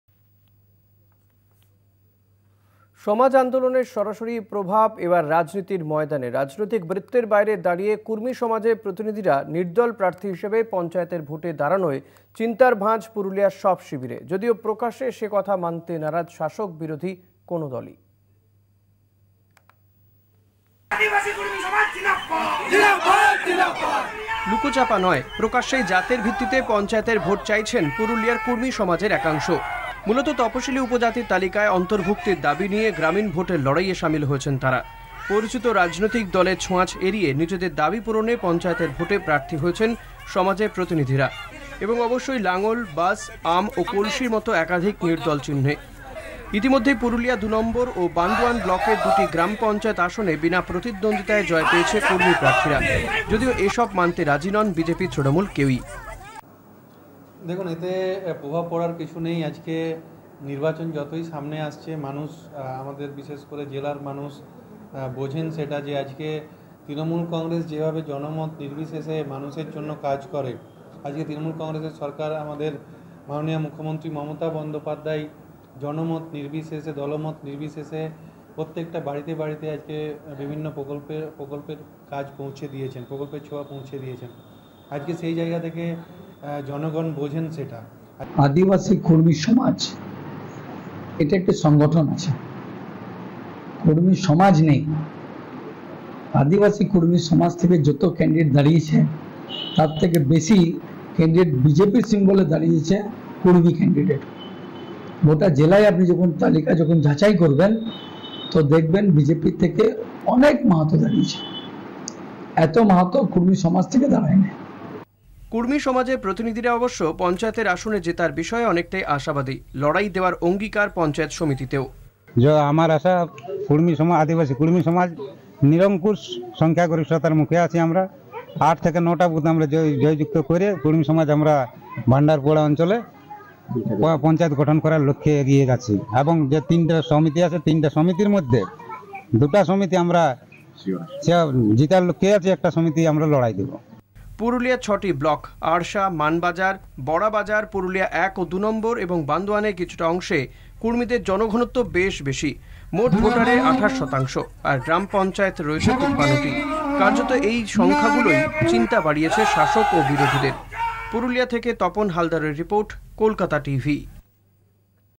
Panchayat Election 2023 | TMC | ভোটার স্লিপ না নেওয়ার অপরাধে তৃণমূল কর্মীর উপর হামলা
website link for this copy :
@Kolkata Tv